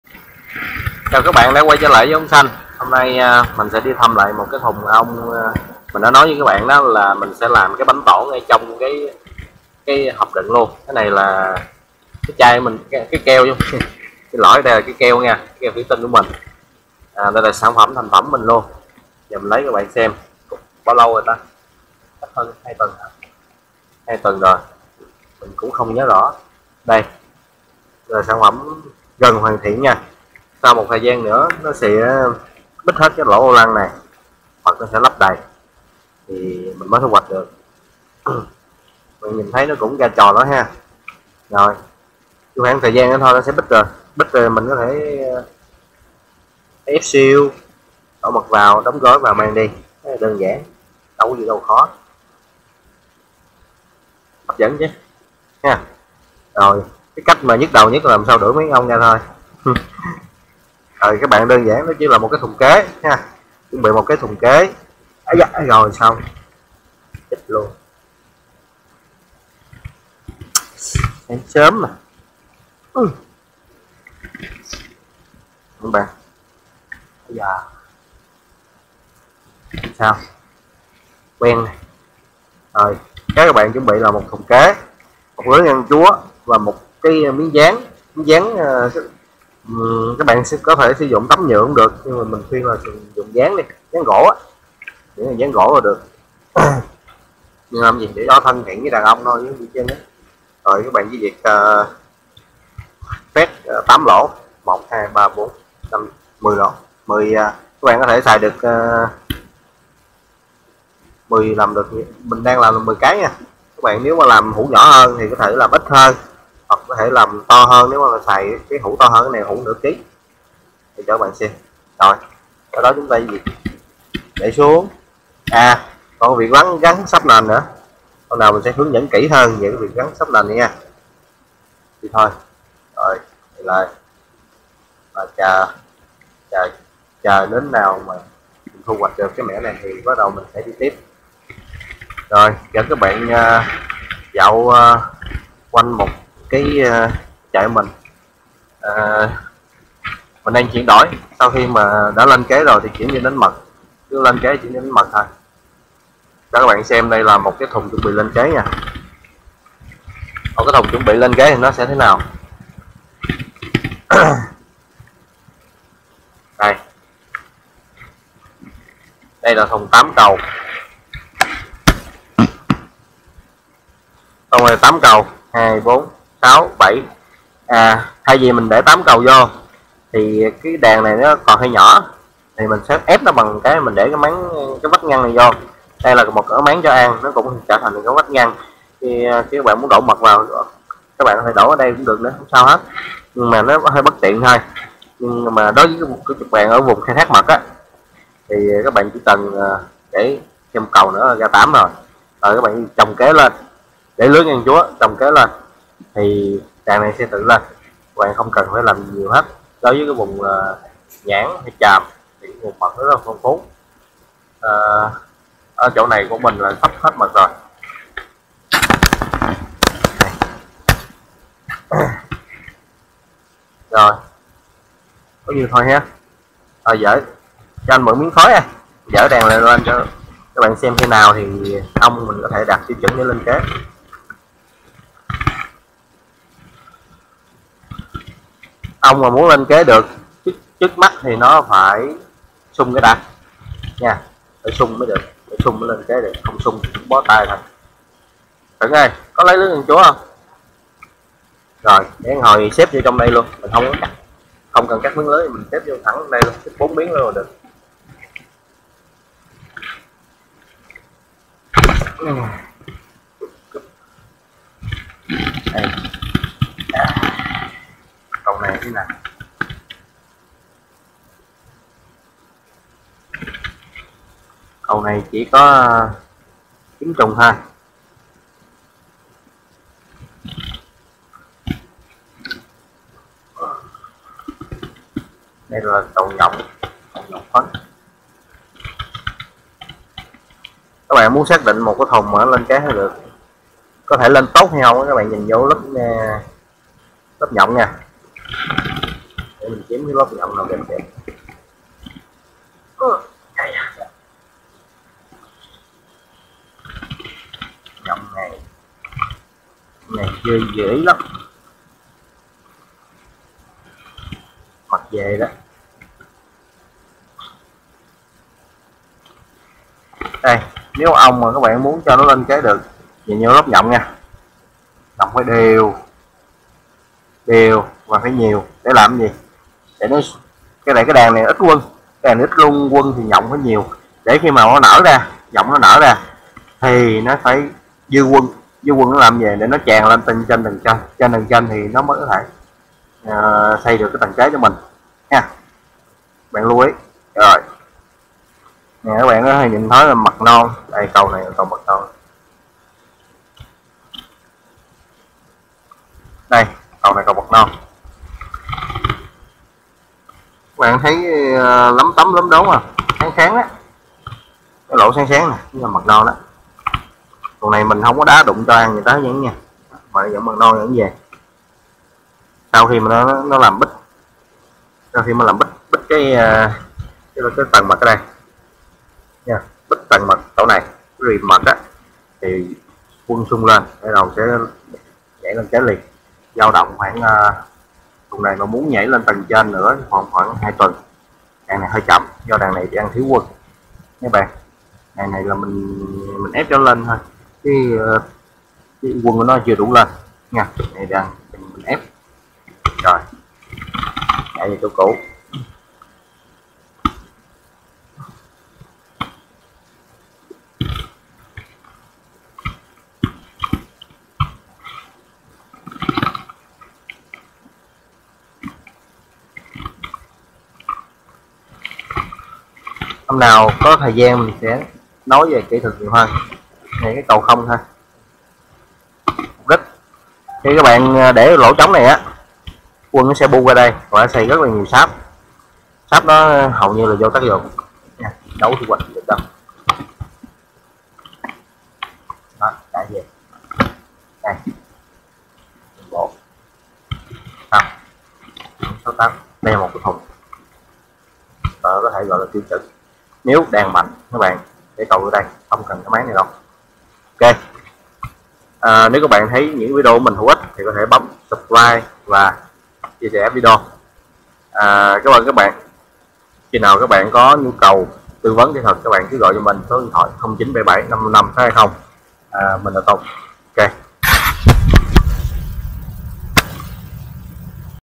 Đàn Ong nhả mật ngay trong hủ thuỷ tinh - Ong Xanh Beekeeping
Cách làm mới cho anh em nào chưa trang bị được bao bì thích hợp, thích hợp với việc kinh doanh hộ gia đình nhỏ lẻ, chỉ cần đưa hủ có gắn sáp vào chờ đợi, chỉ làm được cho anh em nuôi thùng kế nhé!